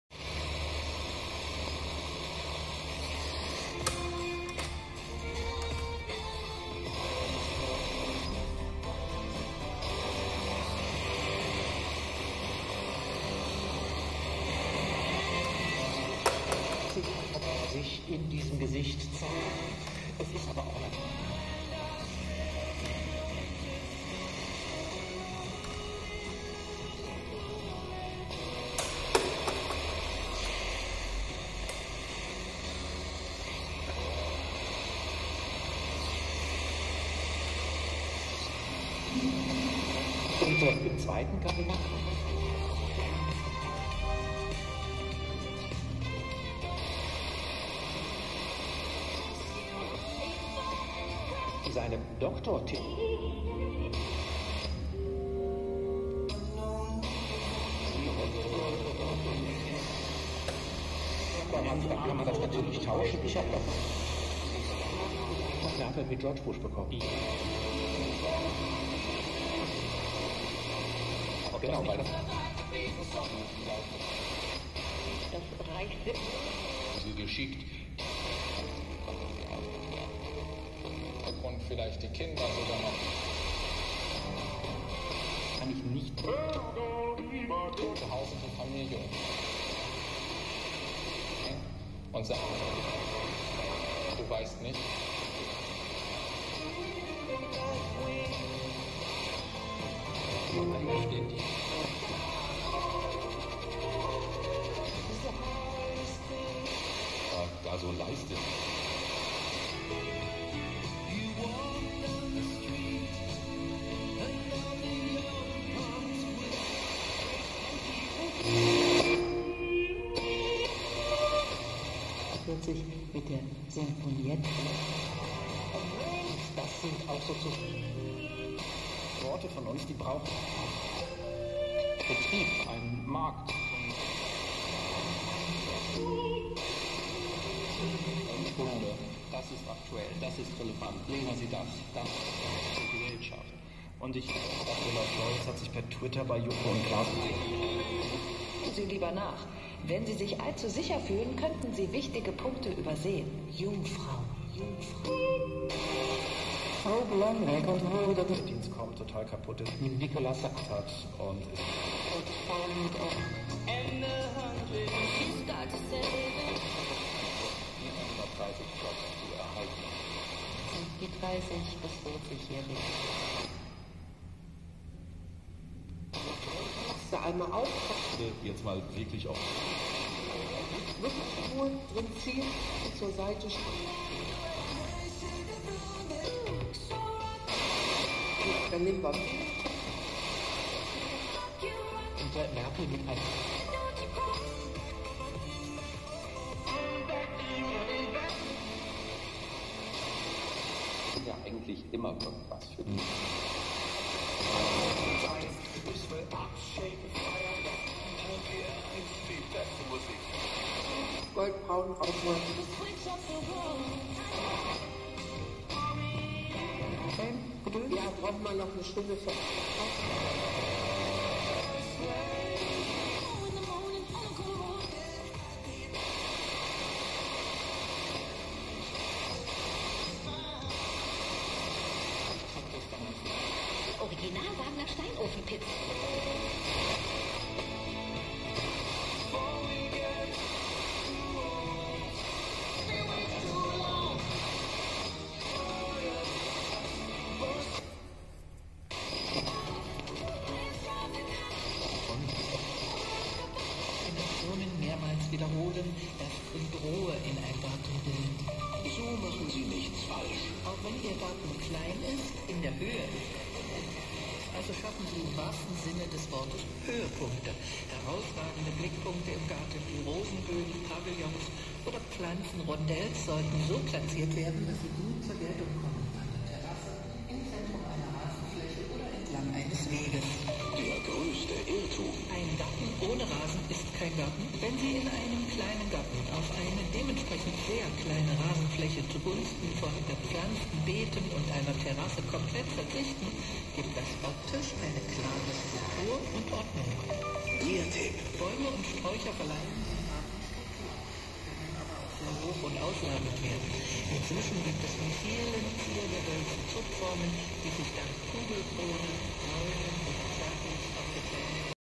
FM bandscan Niederwenigern - Quickscan tropo
Quick FM scan in Hattingen-Niederwenigern. Roadstar TRA2350P (110/53) indoor, telescopic aerial